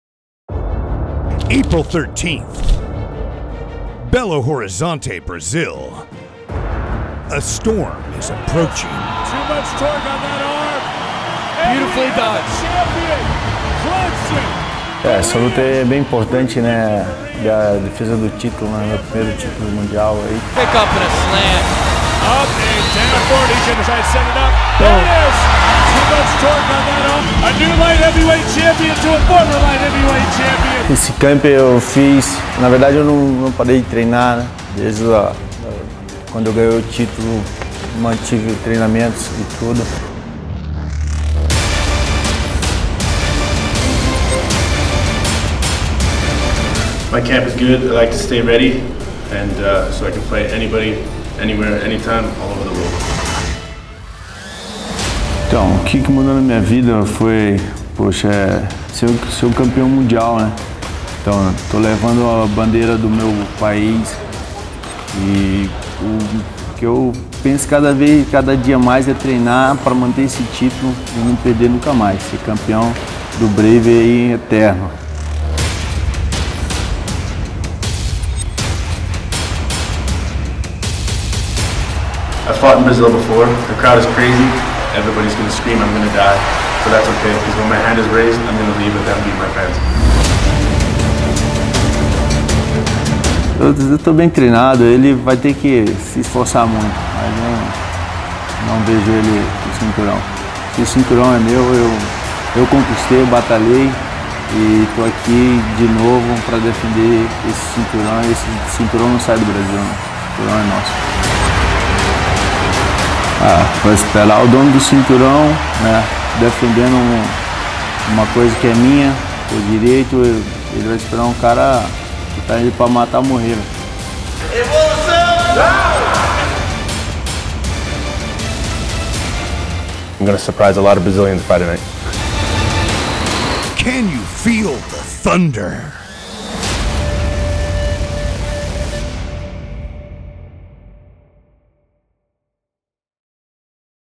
BRAVE CF 11: PROMO - KLIDSON DE ABREU VS MATT BAKERV
Light Heavyweight Gold is on the line at the BraveCF11 co-main event.
The reigning champion Klidson de Abreu defends his title against the challenger "The Riotmaker" Matt Baker.
Storm is approaching. Can you feel the thunder?
Brave 11: Mineiro vs Santiago
13th April, 2018
Belo Horizonte, Brazil